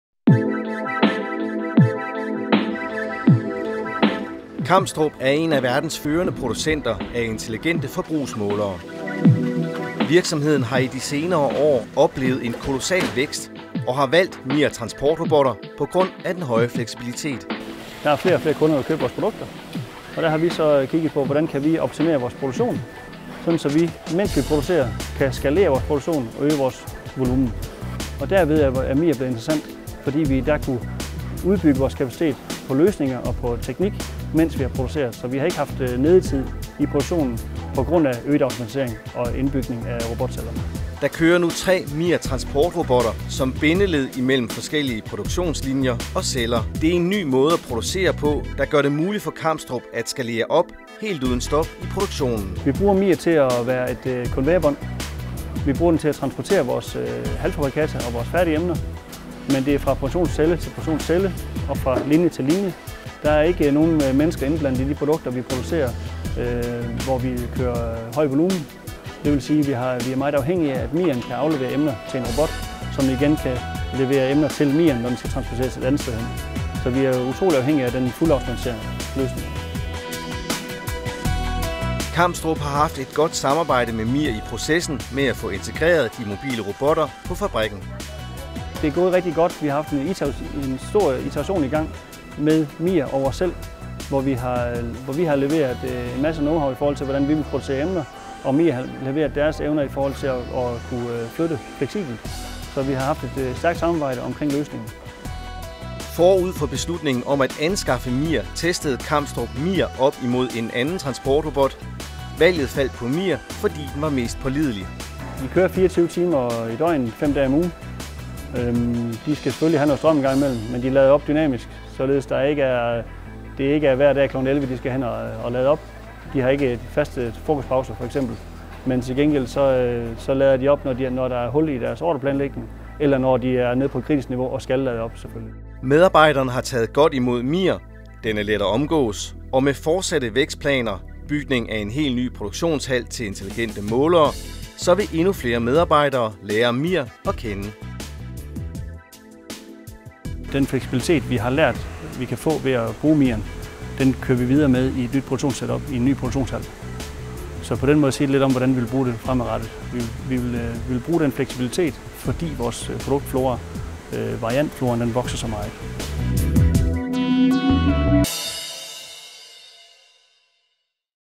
MiR_Kamstrup Case_DK
På ét år har den globale elektronikproducent Kamstrup fordoblet sin produktion helt uden at øge lønomkostningerne.
Opskriften er fleksibel automation og mobile robotter fra Mobile Industrial Robots.